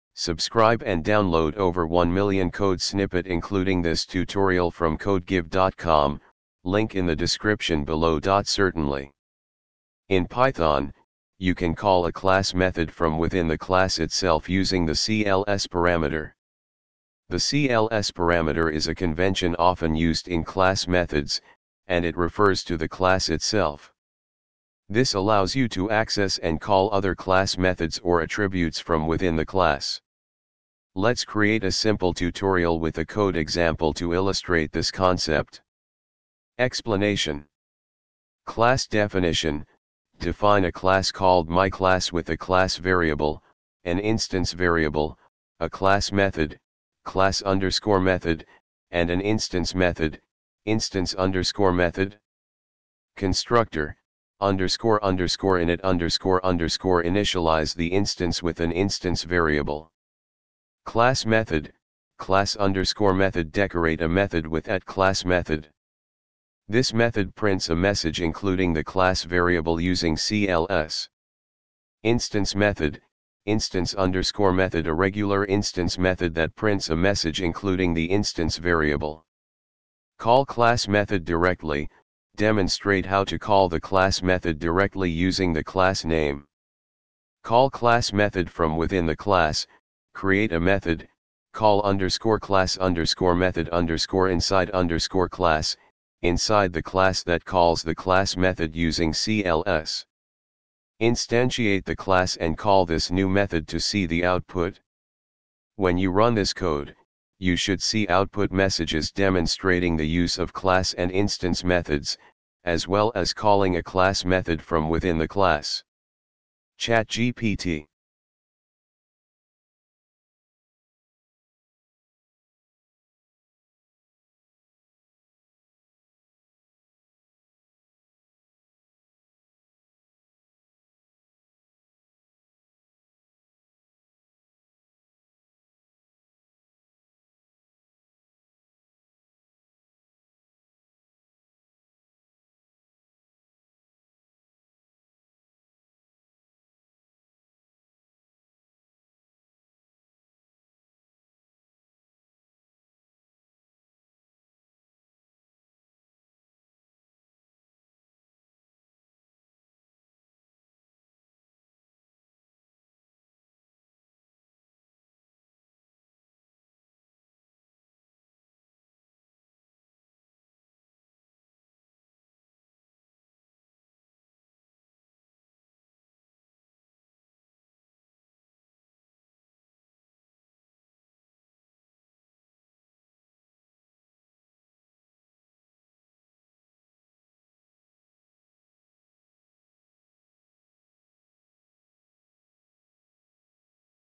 python call class method inside class
Certainly! In Python, you can call a class method from within the class itself using the cls parameter. The cls parameter is a convention often used in class methods, and it refers to the class itself. This allows you to access and call other class methods or attributes from within the class.
Let's create a simple tutorial with a code example to illustrate this concept:
Explanation:
Class Definition: Define a class called MyClass with a class variable, an instance variable, a class method (class_method), and an instance method (instance_method).
Constructor (__init__): Initialize the instance with an instance variable.
Class Method (class_method): Decorate a method with @classmethod. This method prints a message including the class variable using cls.
Instance Method (instance_method): A regular instance method that prints a message including the instance variable.
Call Class Method Directly: Demonstrate how to call the class method directly using the class name.
Call Class Method from Within the Class: Create a method (call_class_method_inside_class) inside the class that calls the class method using cls. Instantiate the class and call this new method to see the output.
When you run this code, you should see output messages demonstrating the use of class and instance methods, as well as calling a class method from within the class.
ChatGPT